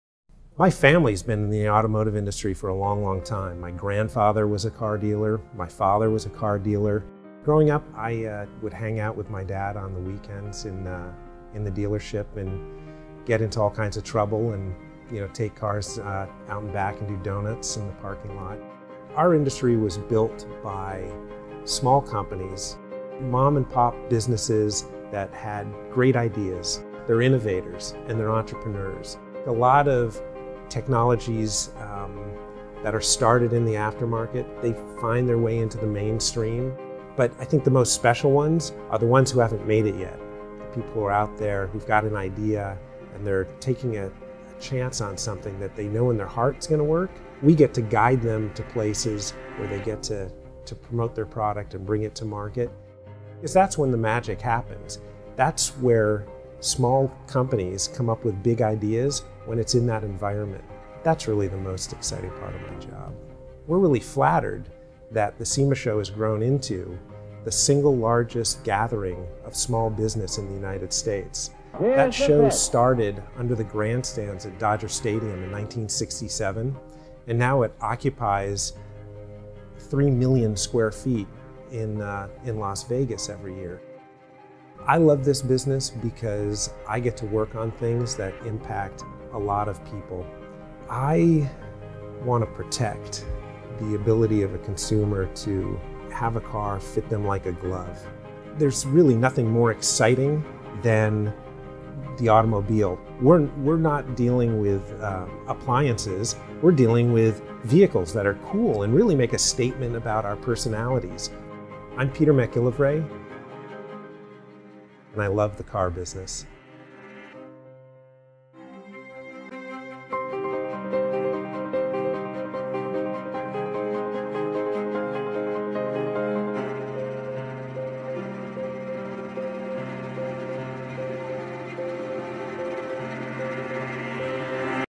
SEMA | Peter MacGillivray | Love the Car Business
With the 2013 SEMA Show right around the corner, we decided to sit down with SEMA Vice President Peter MacGillivray to find out why he Loves the Car Business.